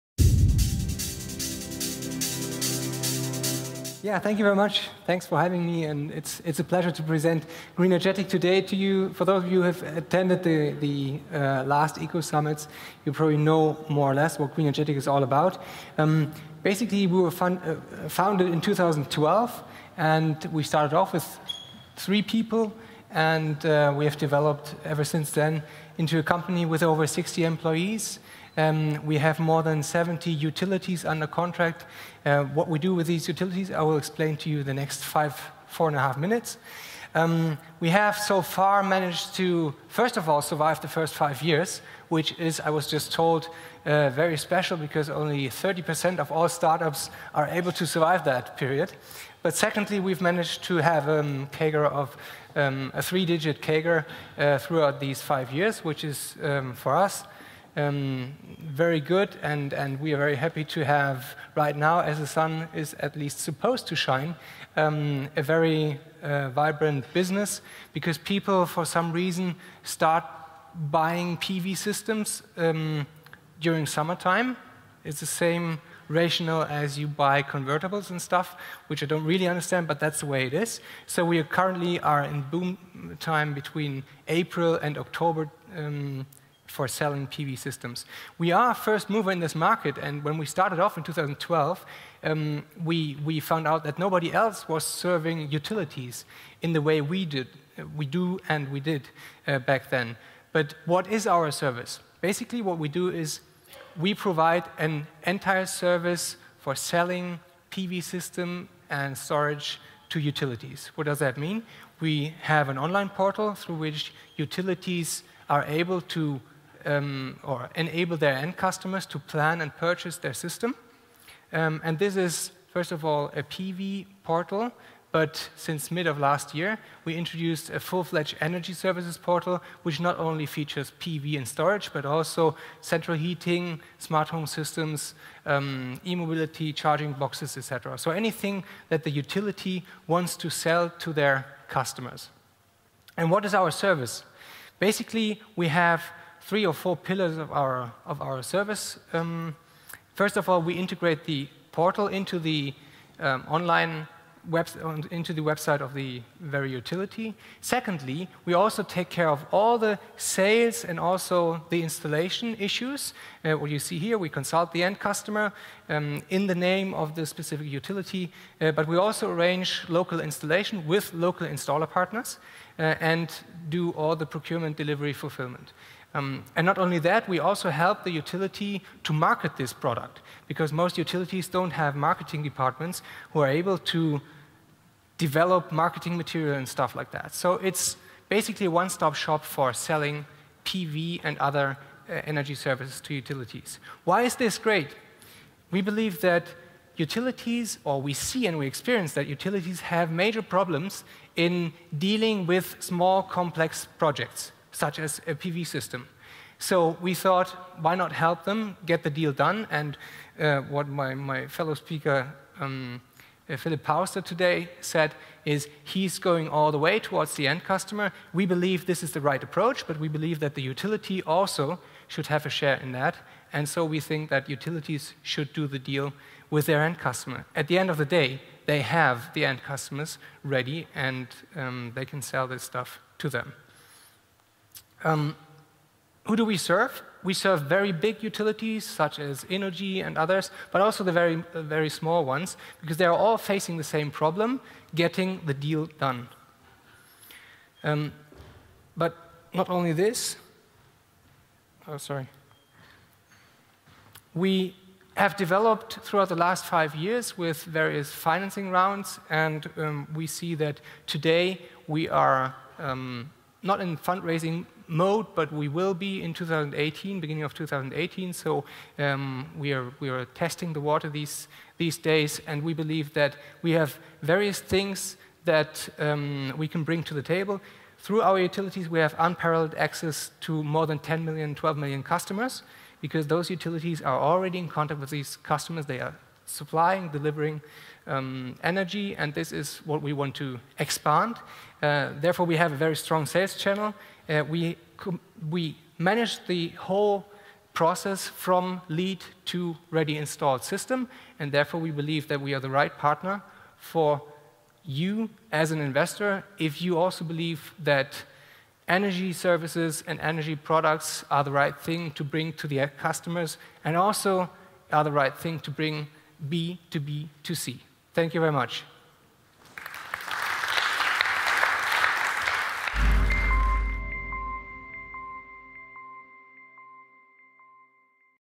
ECO17 Berlin: Florian Meyer-Delpho Greenergetic
Florian Meyer-Delpho pitches Greenergetic at Ecosummit Berlin 2017.